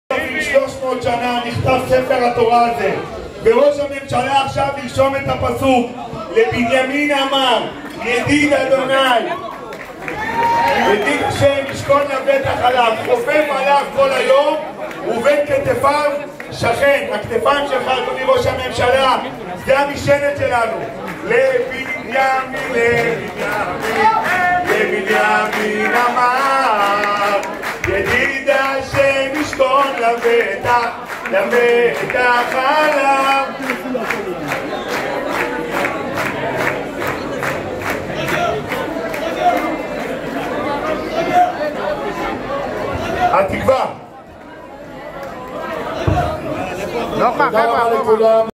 ראש הממשלה בנימין נתניהו בפתח תקווה
צפו בראש הממשלה כותב אות בספר התורה של עם ישראל. ספר בו לוקחים חלק כל יהודי ויהודי מעם ישראל.

לכתיבת אות בספר התורה של עם ישראל לחצו על הקישור